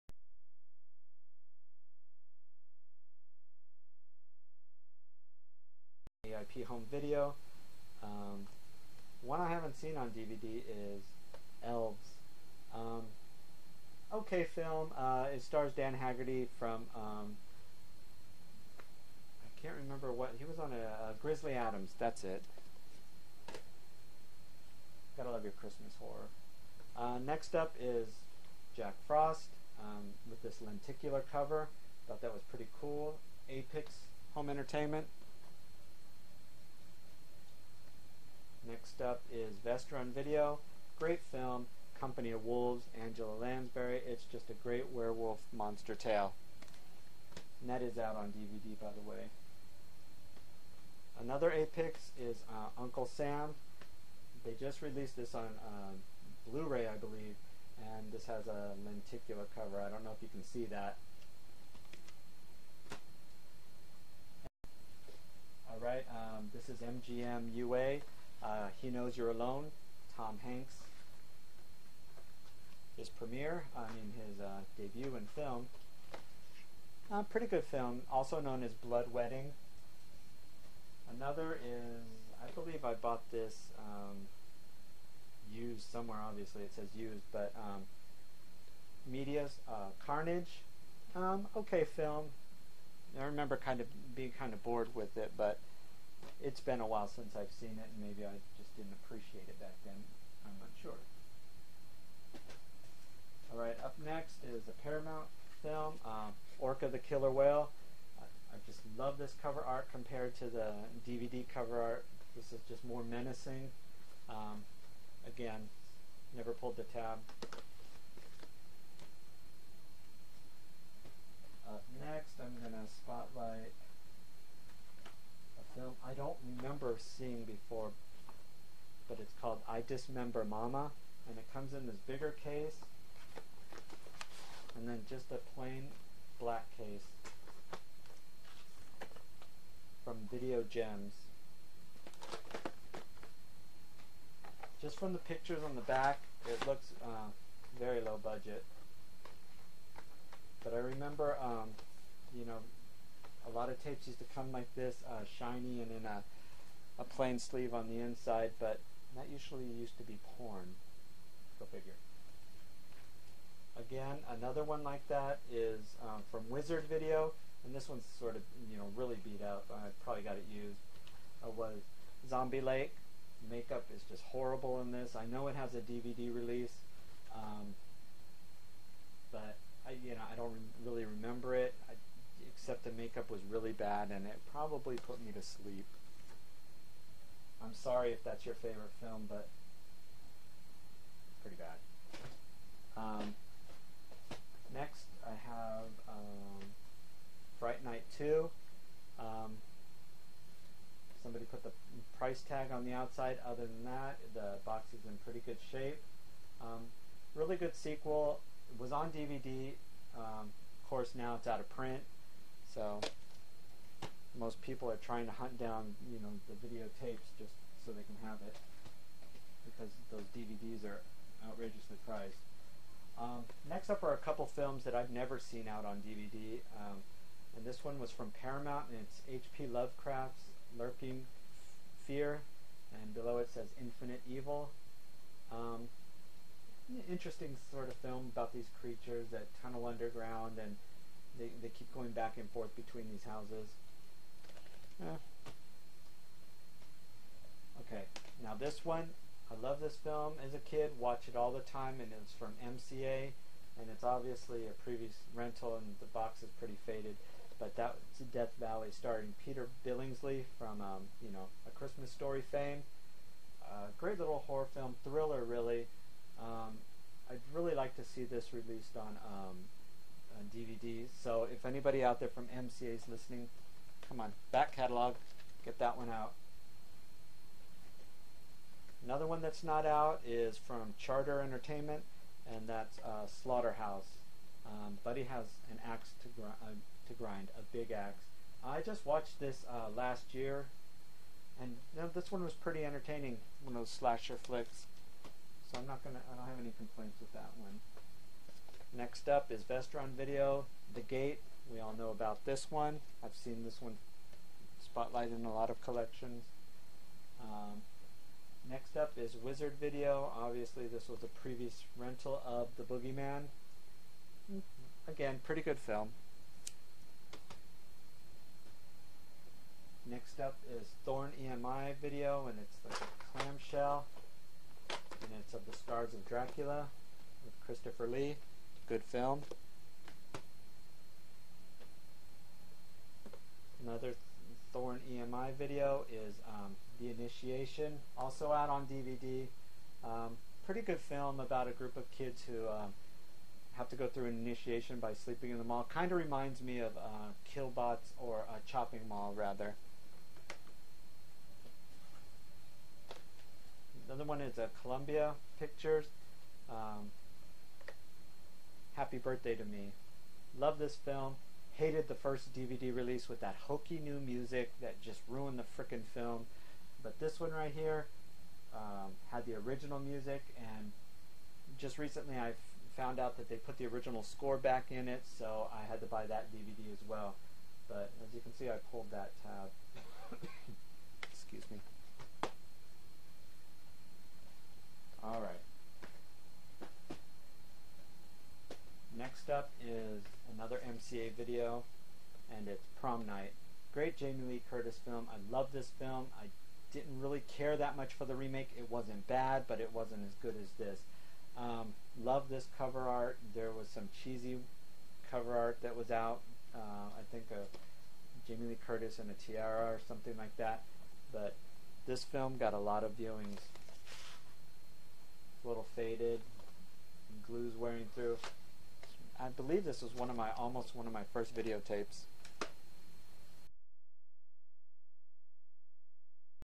Horror Video Collection (Part 2)
This is the second installment of my horror video tapes I found rummaging around in the garage.  Enjoy.